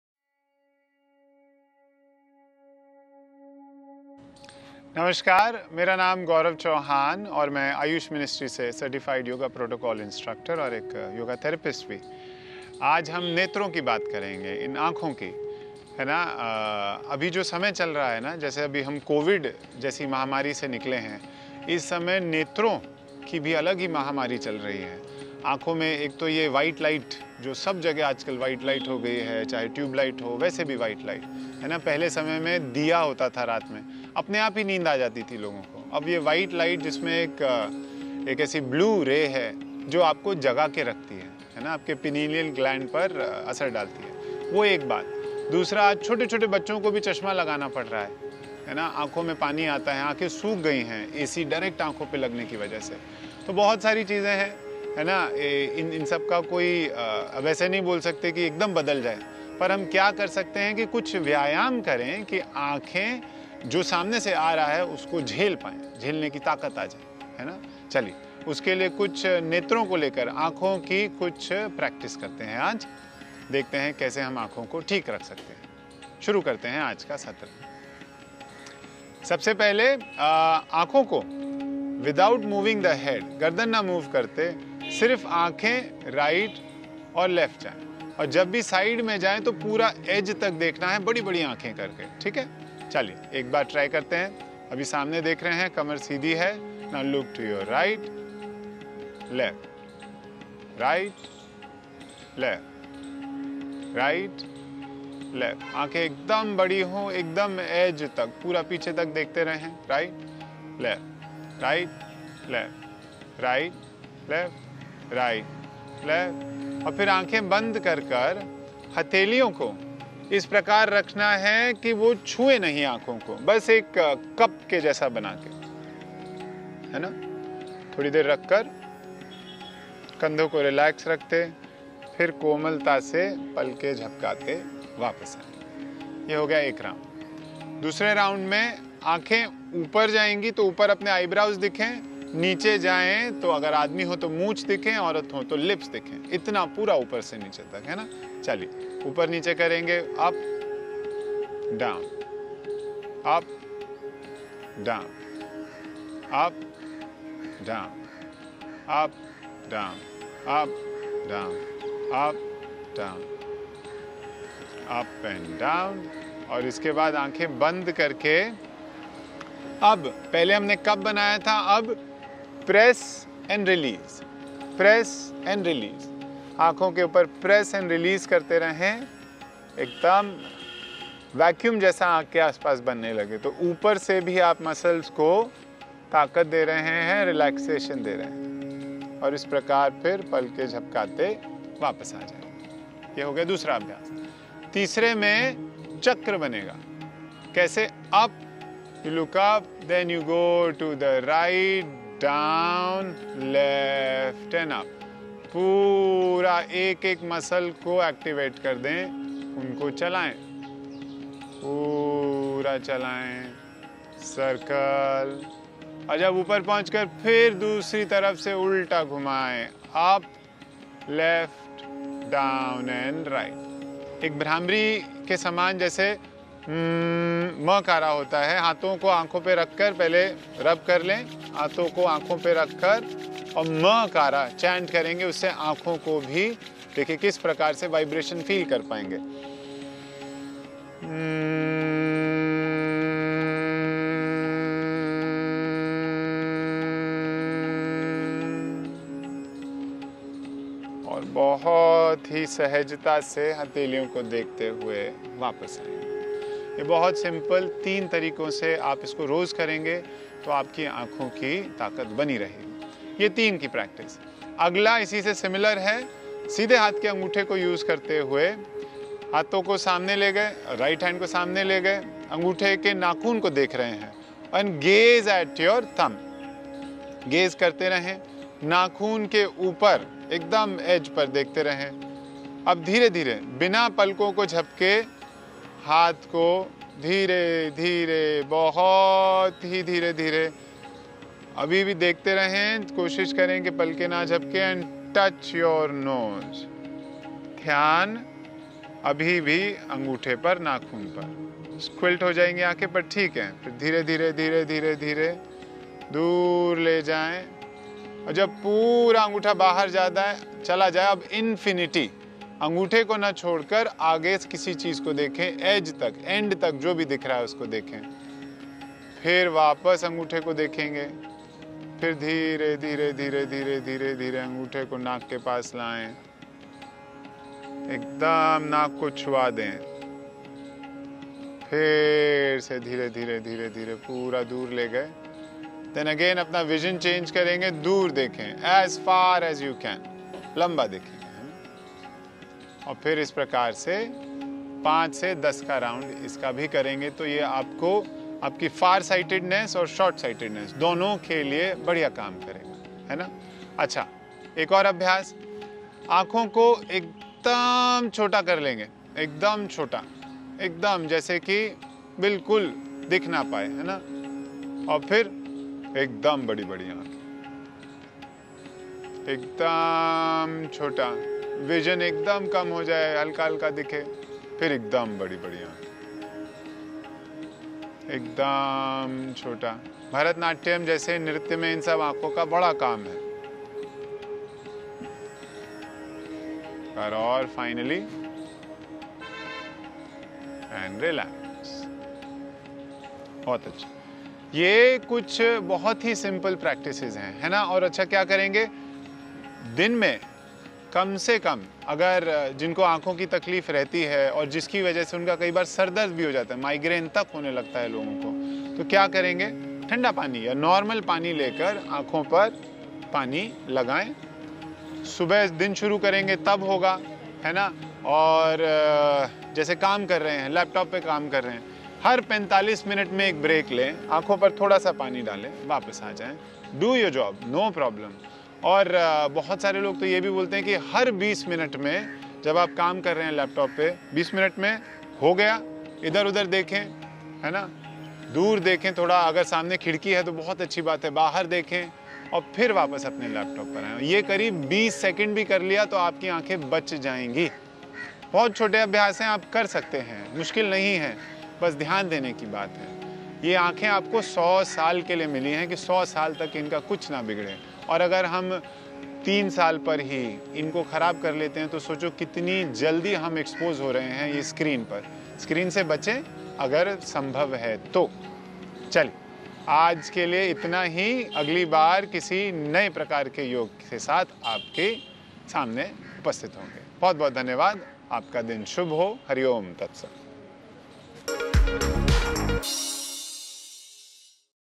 Yoga for good Eyesight: आंखों को स्वस्थ रखने के लिए करें ये आसान सी योग क्रियाएं | NBT Life
Eye yoga has great importance in yoga, and it is also very important to take care of the eyes. So in this video, our yoga expert Gaurav Chauhan will tell how by doing yoga you can keep your eyes always healthy.

योगा में आंखों के योगा का बहुत महत्व है, और आंखों का ध्यान रखना भी बहुत ज़रूरी है. तो इस वीडियो में हमारे योगा एक्सपर्ट गौरव चौहान बताएंगे कि किस तरह से योगा करके आप अपनी आंखों को रख सकते हैं हमेशा स्वस्थ . देखें पूरा वीडियो.